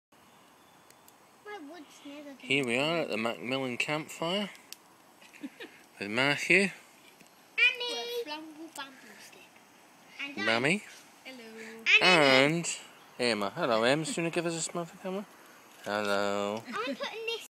Macmillan's do bushcraft and campfire
Having started a barbecue and campfire with a firesteel we enjoyed burgers and ho-cho